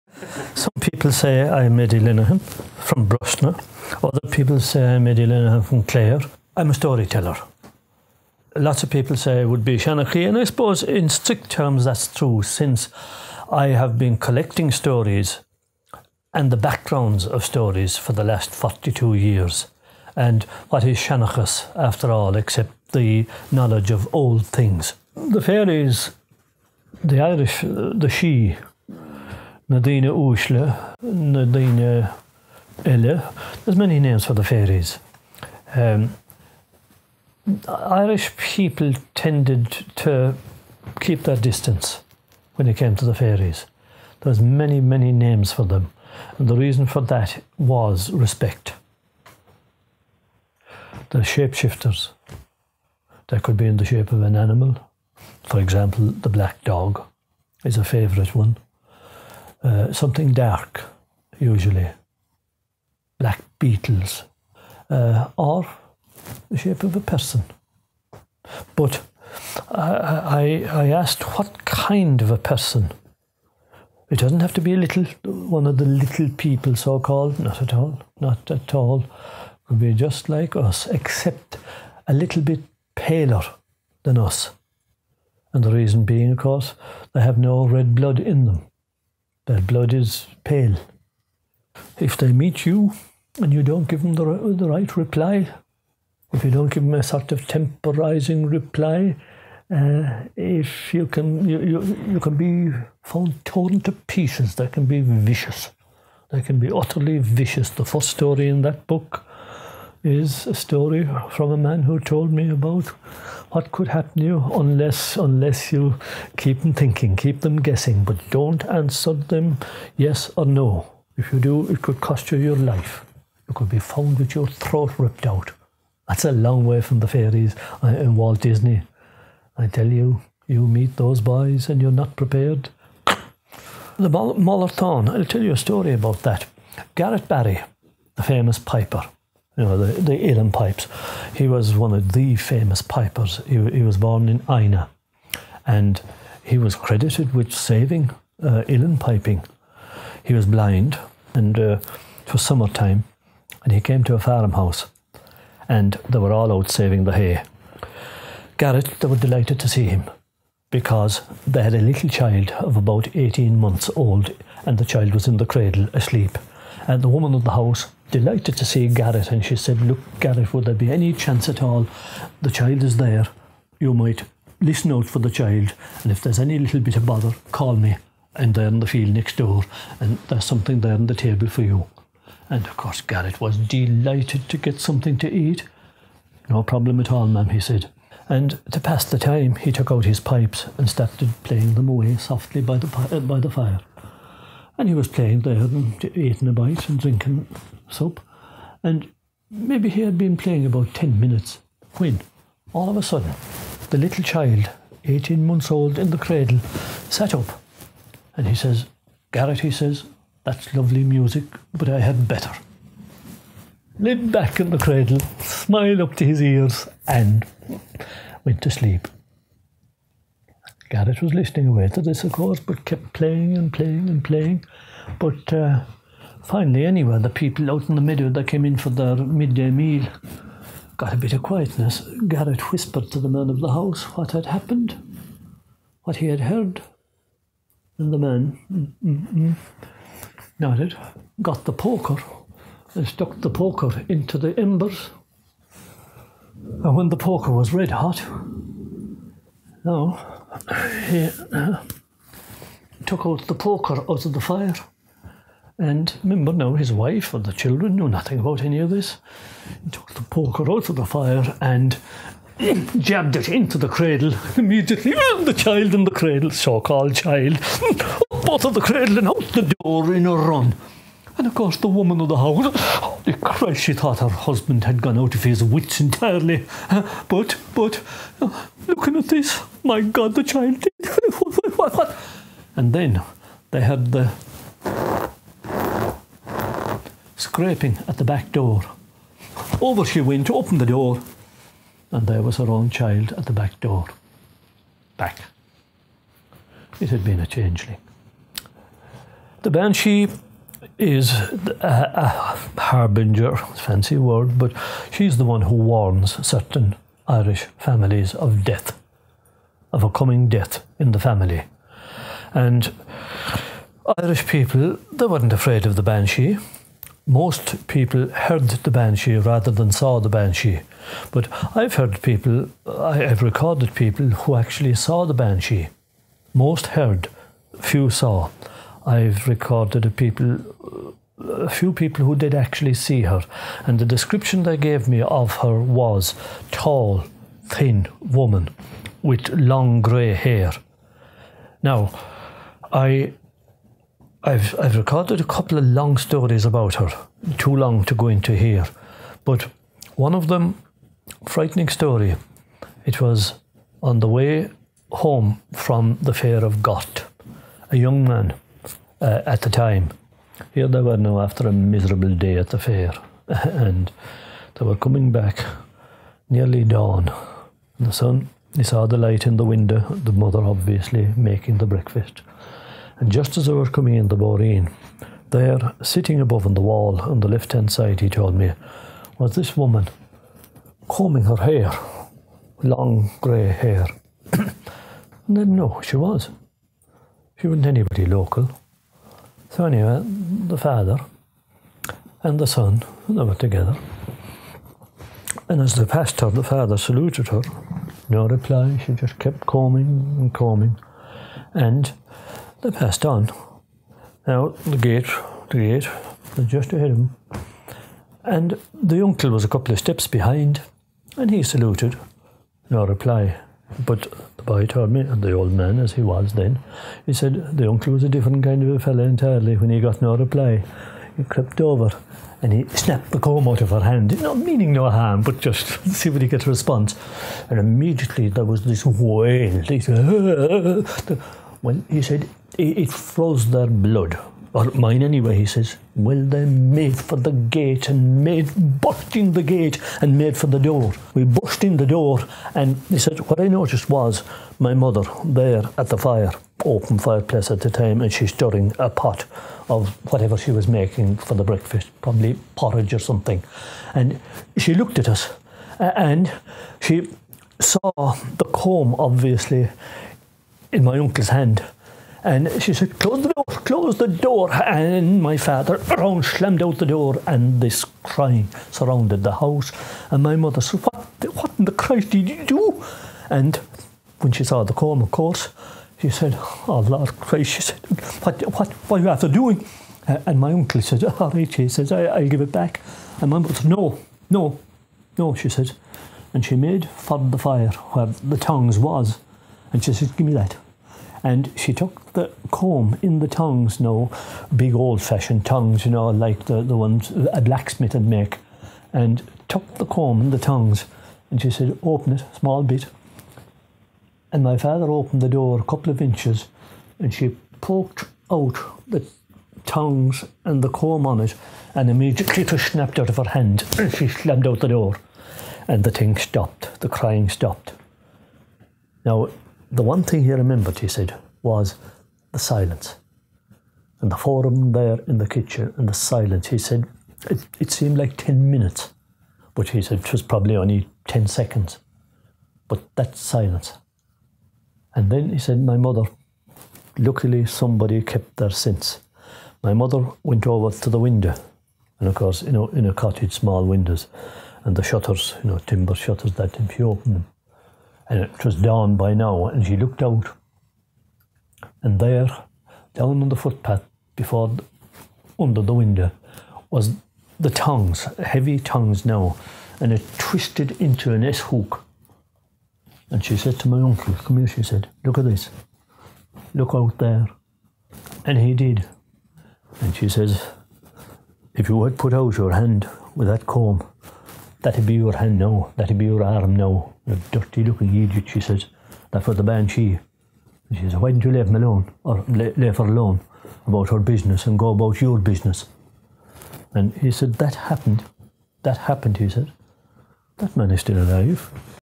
Episode 1 - Irish Folklore - Eddie Lenihan
In our first episode we speak to world famous storyteller Eddie Lenihan about what makes Irelands Folklore so unique from the rest of the world.